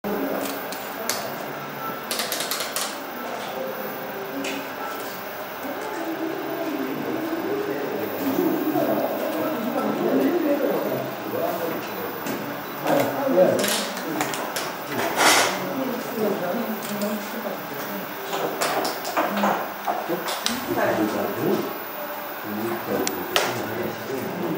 Boys repairing CRT TVs at the Children's Palace in Kaesong, North Korea
The cathode ray tube still seems to not be 100% dead as of 2014 - here it seems to be used by boys practising electrical circuits.